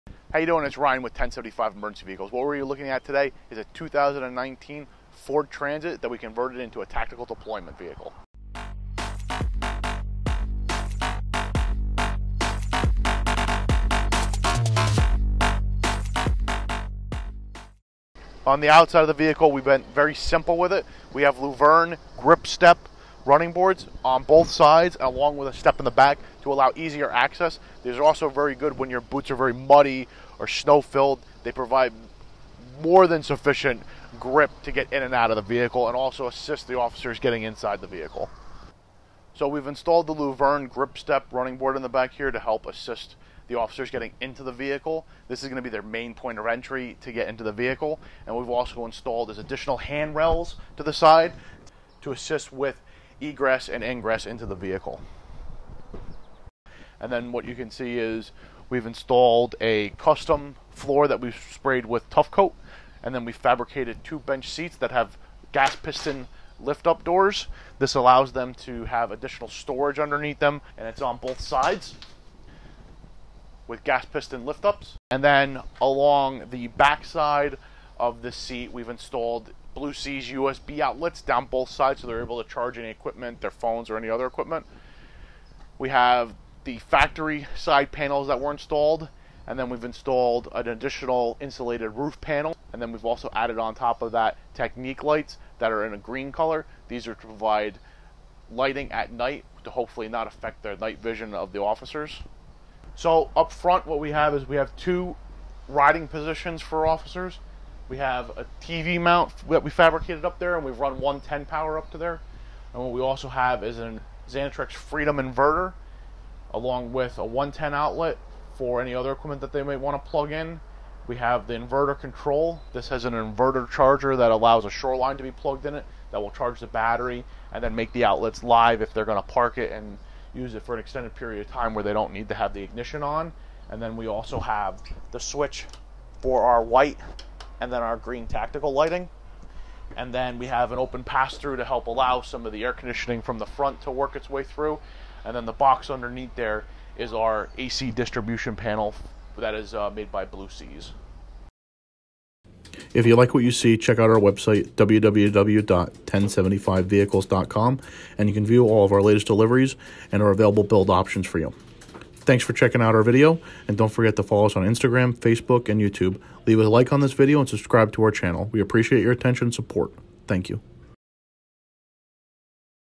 SWAT VAN | Police Vehicles Upfitter | Special Forces
Check out this SWAT Van featuring light for night vision, custom SWAT insert and much more!